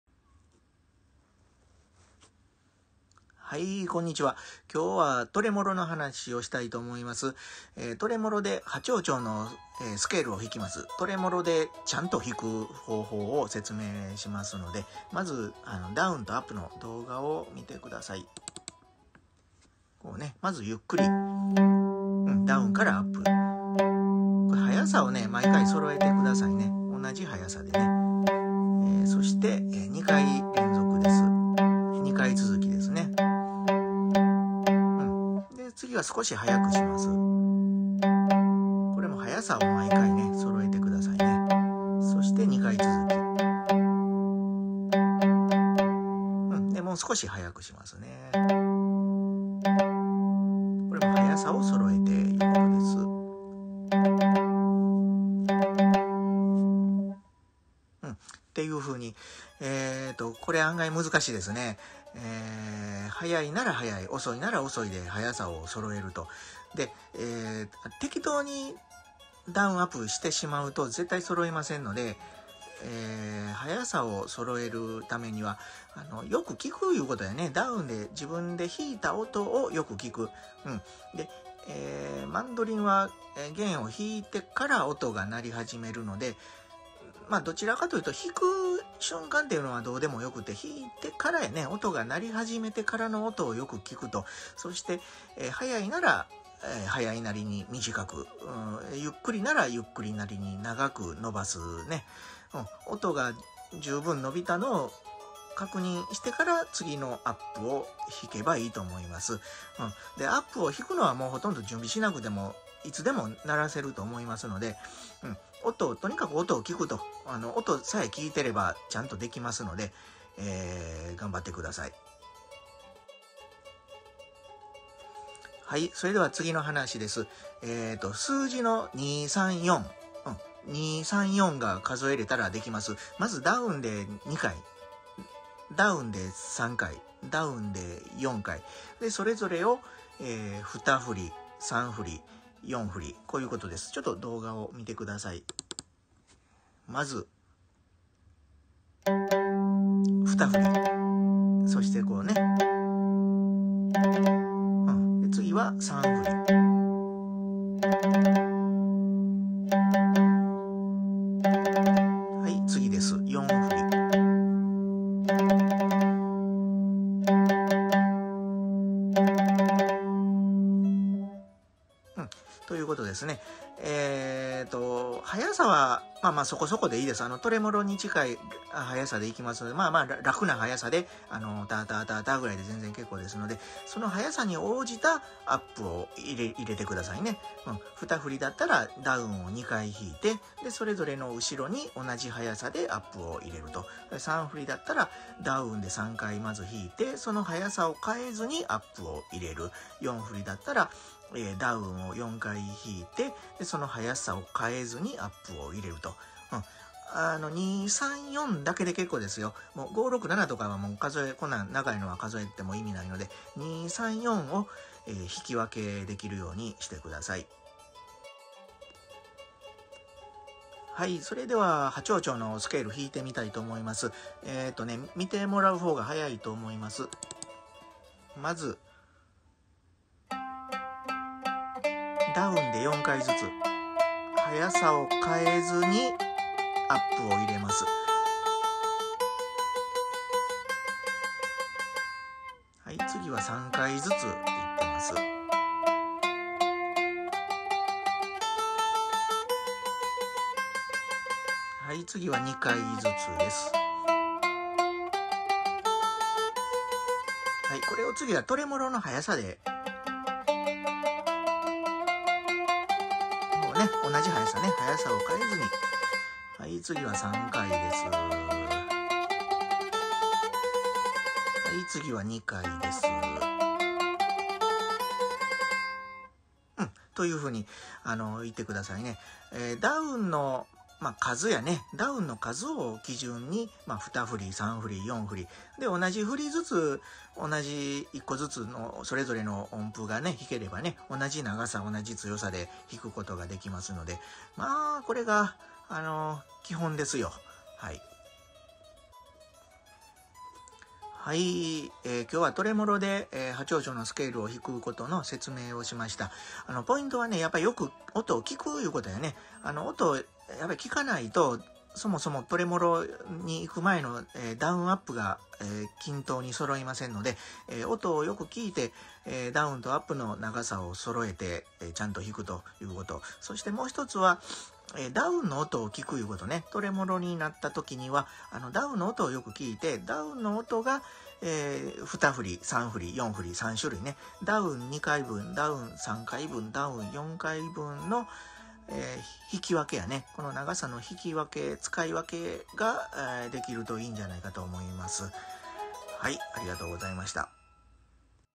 【マンドリン初心者】分かりやすい弾き方19 トレモロの基本
トレモロでハ長調のスケールを弾きます。

まず、ダウンとアップをゆっくり練習します。均等に鳴るように気をつけます。

次はダウンだけで数を決めて練習します。
2,3,4回でOKです。
同じ速さでアップを加えます。

ダウンで弾いた音をよく聴くことがポイントです。

マンドリン
大西功造
mandolin